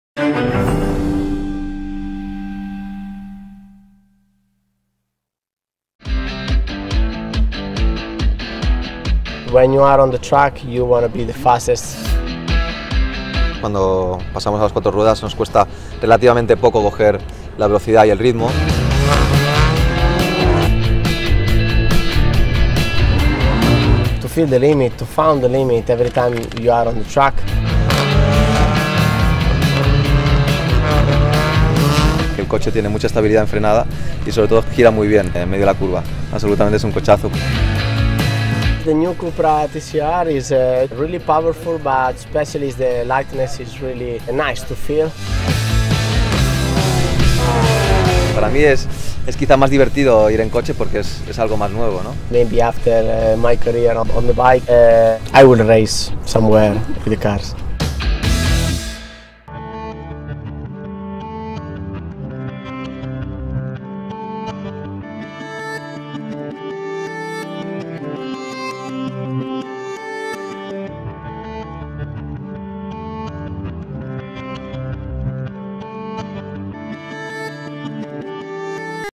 DOVIZIOSO AND LORENZO, AT THE WHEEL OF THE CUPRA TCR.
We’re used to seeing them racing on their motorcycles and taking corners with their knee grazing the asphalt. Whether it’s on two or four wheels, Ducati Corse MotoGP racers and CUPRA ambassadors Andrea Dovizioso and Jorge Lorenzo are motoring enthusiasts and didn’t hesitate in exchanging vehicles for a day. The challenge: take the CUPRA TCR for a drive following the instructions of CUPRA team racer Pepe Oriola and brand ambassador Jordi Gené.

- From handlebars to steering wheel: “You want the same thing whether you’re driving a car or riding a motorcycle – when you’re on the track you want to be the fastest. The difference is that the car makes you feel safer it gives you more stability”, says Andrea Dovizioso as he walks over to the CUPRA TCR on the track of the Castellolí circuit. Jorge Lorenzo adds: “When riders make the switch to four wheels it doesn’t take us long to gather pace and pick up speed”. With the same enthusiasm, they both put on their helmet and start the engine to check the engine response – in just 5.2 seconds the CUPRA TCR reaches 100 km/h.

- Curves that test their mettle: Both riders agree that although they don’t have to lean their body into the asphalt, they feel the same adrenaline rush. “The CUPRA TCR is extremely powerful, I love how it feels to drive“, declares Andrea as he puts the 2.0 TSI engine’s 350 hp output to the test on the track. “It’s very agile and allows for aggressive cornering”, he points out. “It handles really well in the curves you can feel how the car responds to your steering. And it has a lot of stable composure under hard braking”, says the three-time MotoGP world champion.